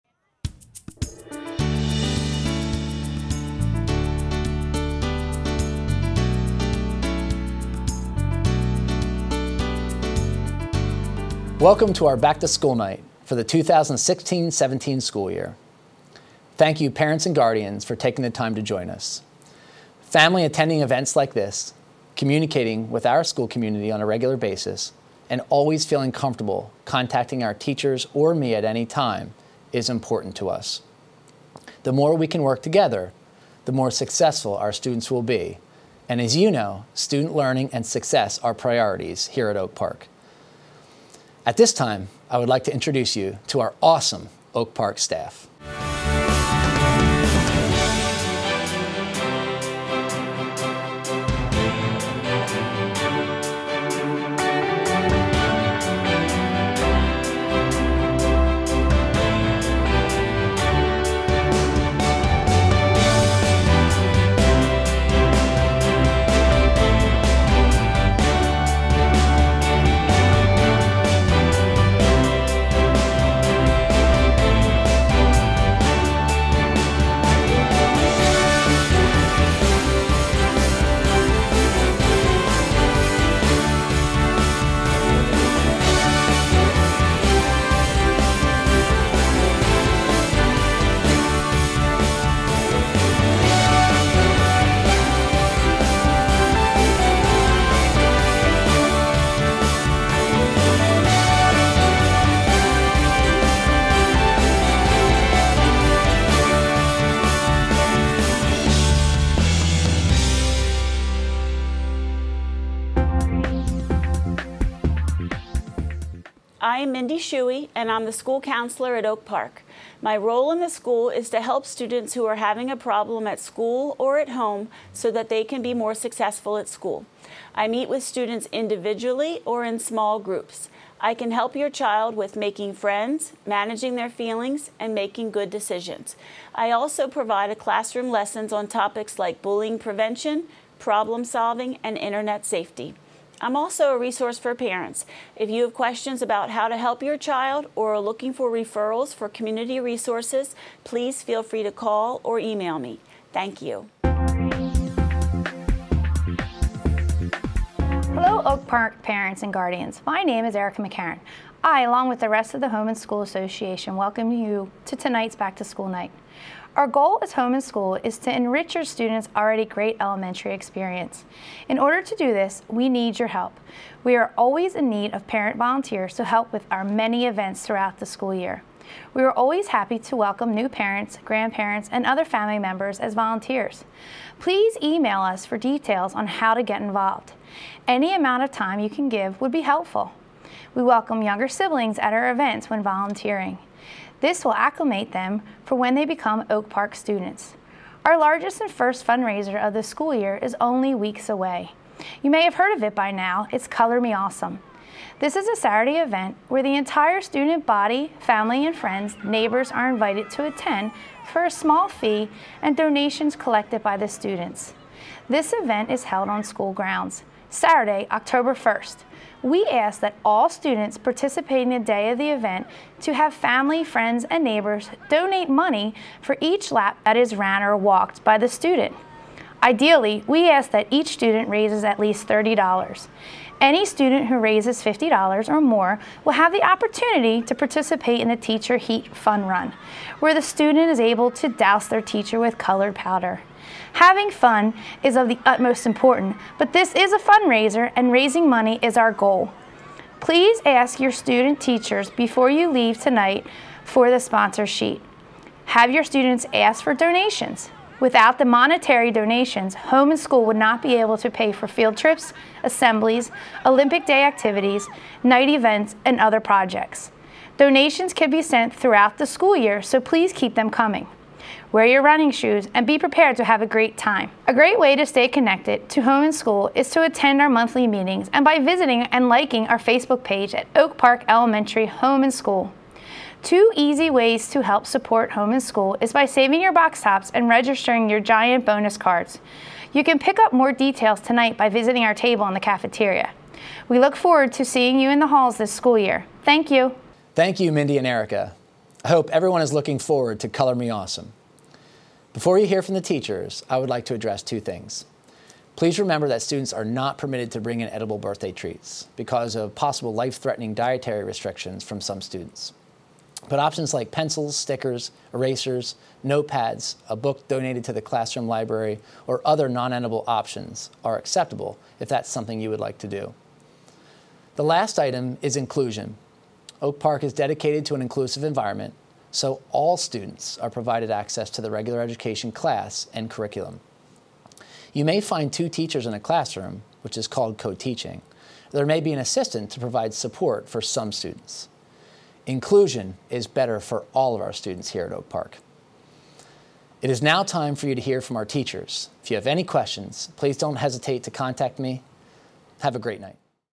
Oak Park Elementary Back to School Night 2016
Welcome back to the 2016-17 school year at Oak Park Elementary School.